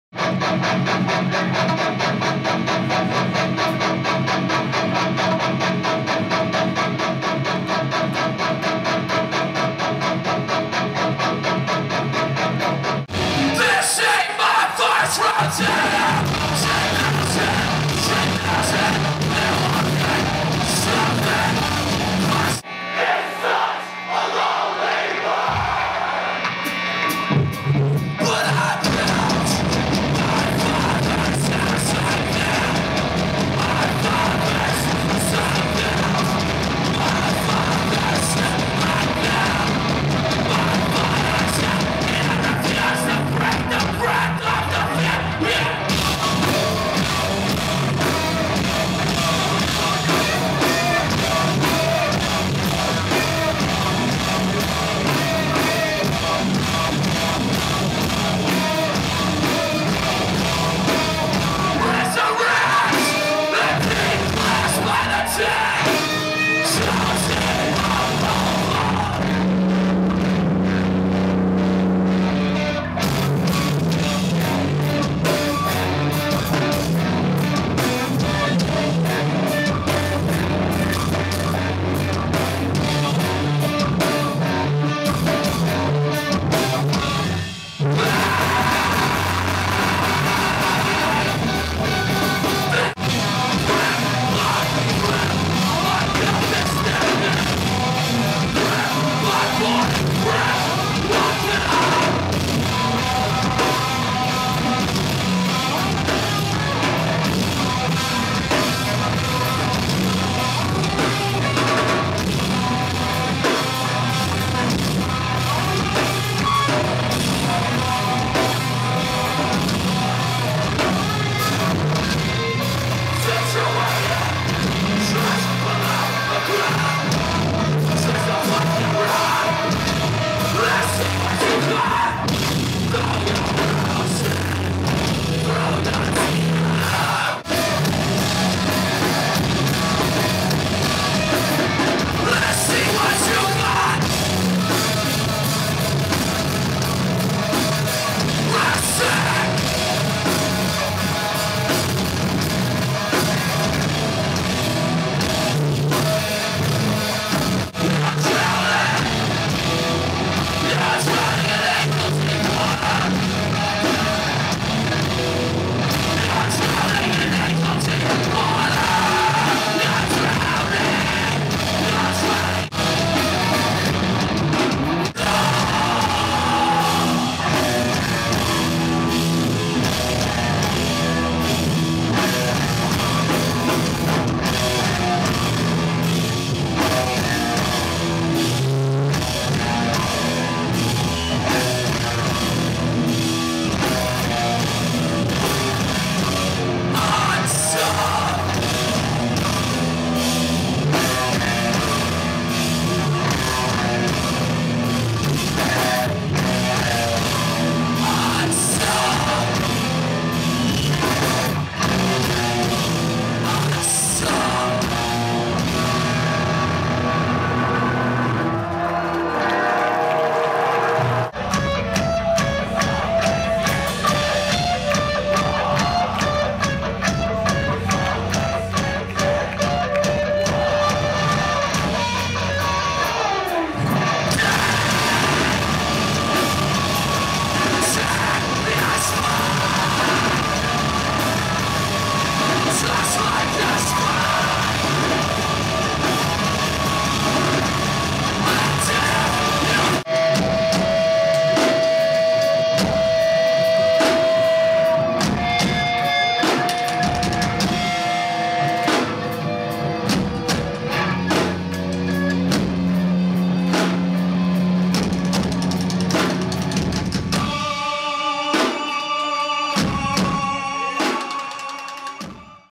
The Chariot - Live at Blender Theater NYC
This is The Chariot playing at the blender theater in NYC. CH CHECK IT.